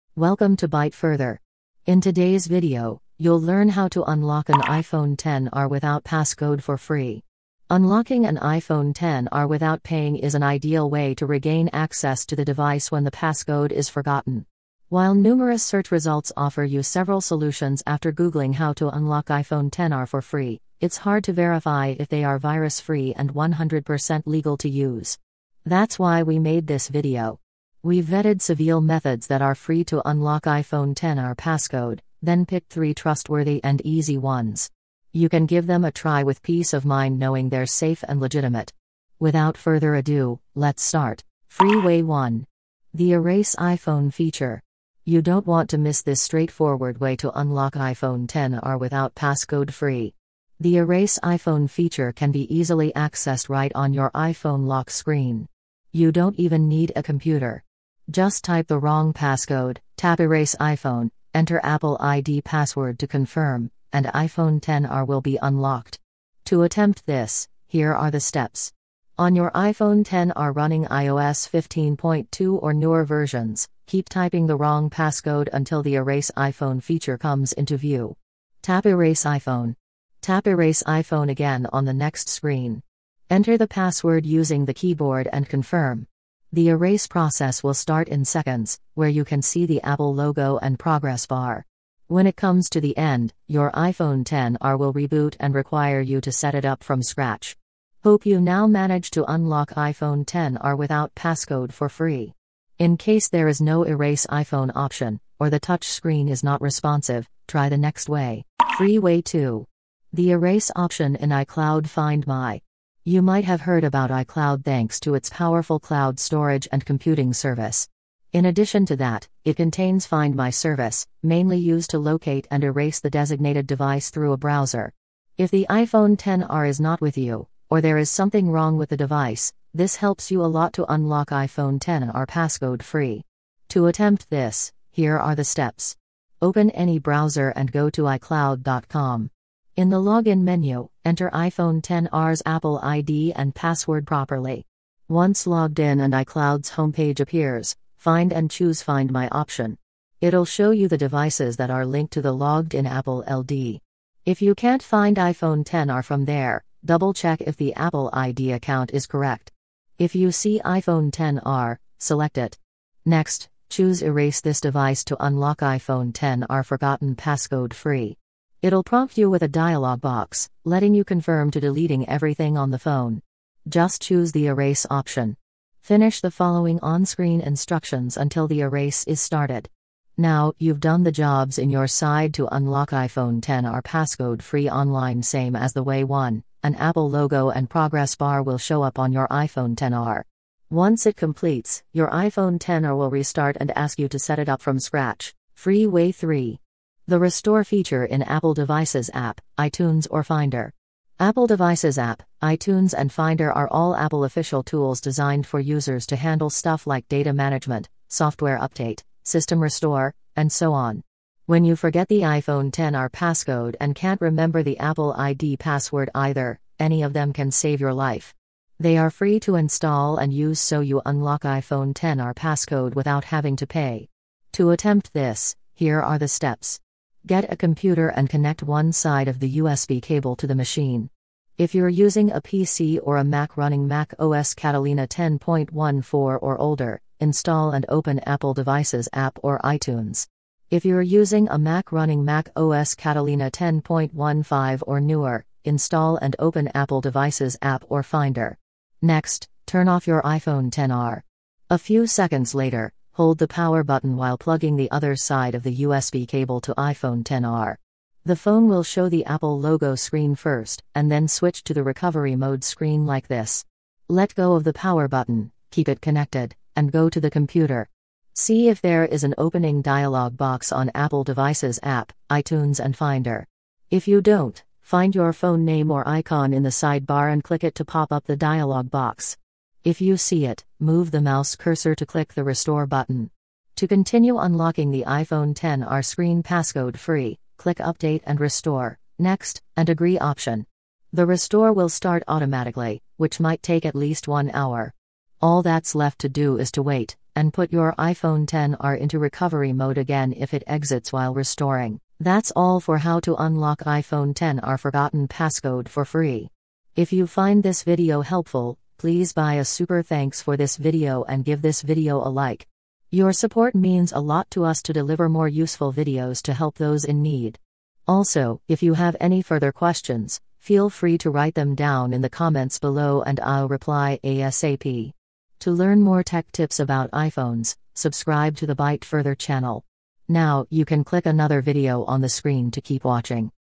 3 Free Ways to Unlock iPhone XR Passcode (A Step-by-Step Guide)
In today's video, you'll learn how to unlock an iPhone XR without passcode for free. Unlocking an iPhone XR without paying is an ideal way to regain access to the device when the passcode is forgotten. While numerous search results offer you several solutions after googling how to unlock iPhone XR for free, it's hard to verify if they are virus-free and 100% legal to use. That's why we made this video. We've vetted seveal methods that are free to unlock iPhone XR passcode, then picked three trustworthy and easy ones.

You can give them a try with peace of mind knowing they're safe and legitimate. Without further ado, let's start.

On this video:

0:41 Free way 1. The Erase iPhone feature.
1:48 Free way 2. The erase option in iCloud Find My.
3:15 Free way 3. The Restore feature in Apple Devices app, iTunes or Finder.

Hope this video can help you unlock iPhone XR without passcode for free.